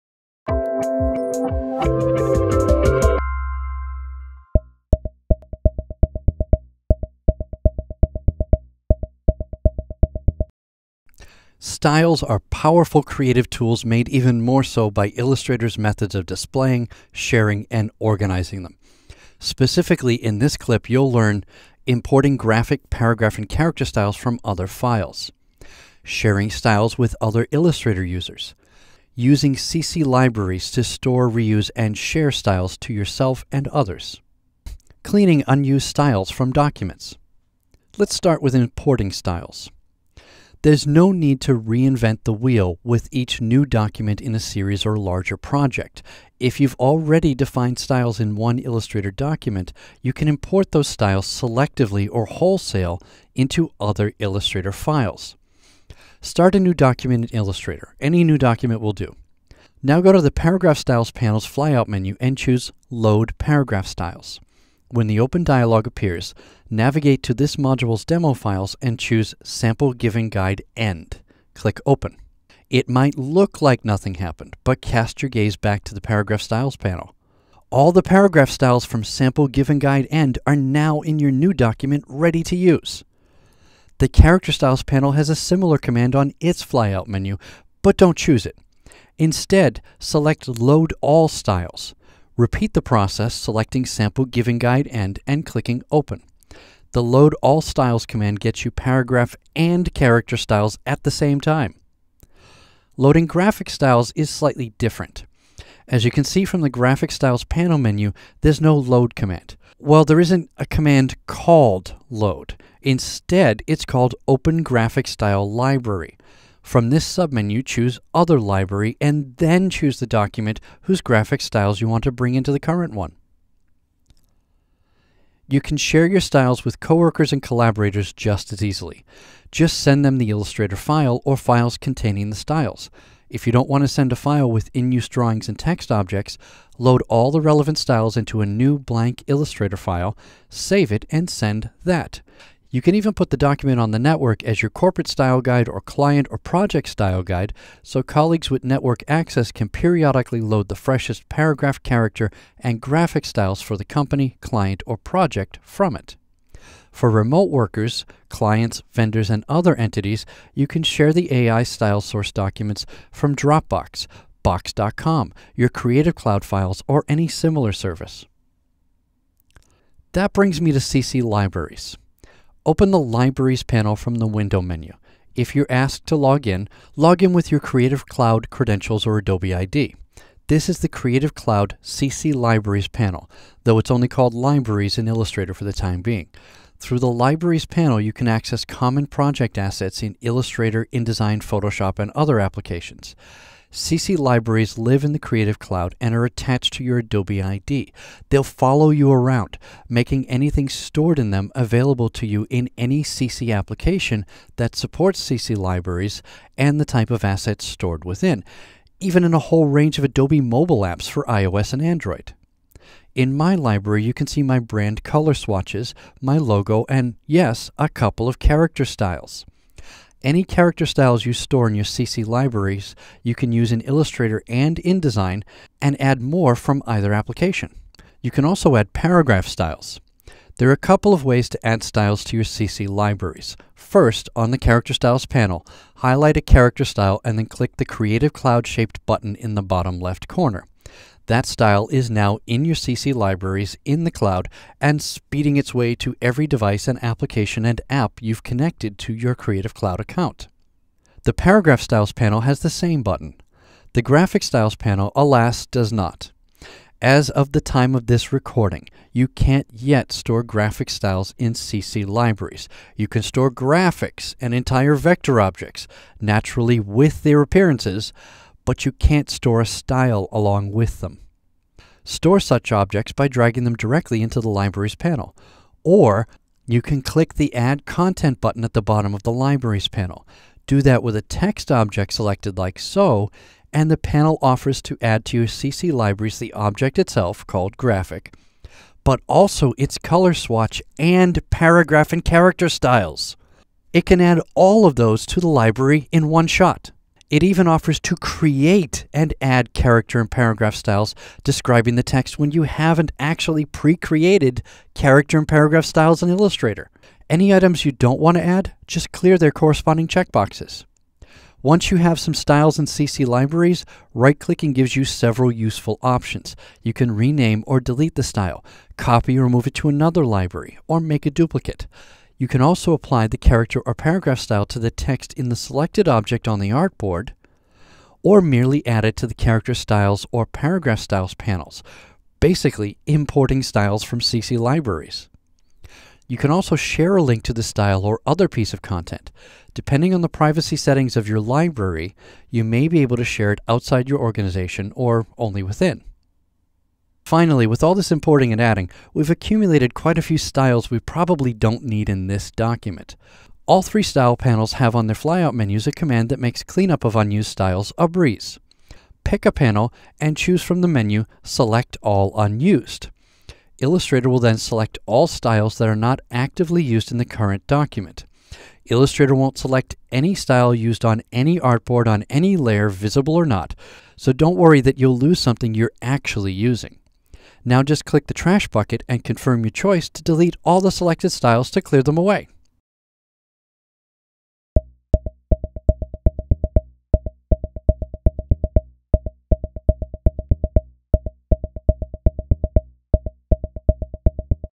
Working with Styles Effectively in Adobe Illustrator
In this tutorial clip you will learn efficient paragraph and character style management and use techniques in Adobe Illustrator CC, including:
- Importing Graphic, Paragraph, and Character Styles from Other Files
- Sharing Styles with Other Illustrator Users
- Using CC Libraries to Store, Reuse, and Share Styles to Yourself and Others
- And, Cleaning Unused Styles from Documents

Excerpted from Pariah Burke's comprehensive Pluralsight video course, Illustrator CC Typography